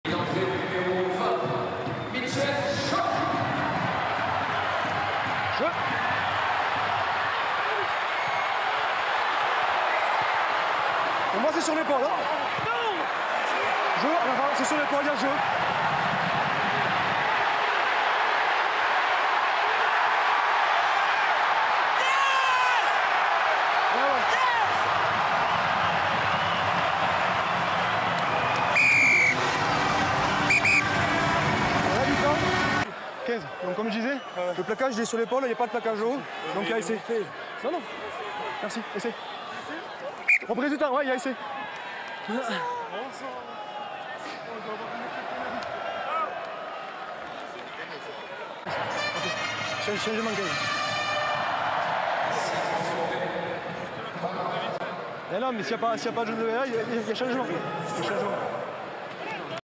TOP 14 - L'image de la J23 : L'éloge de la vitesse
📢Le train TGV (Teddy à Grande Vitesse) numéro 14 en provenance du
Racing 92 va entrer en gare de la Paris La Défense Arena ! Éloignez-vous de la bordure du quai s'il vous plaît 🚄

○ Myrugby : myrugby.lnr.fr/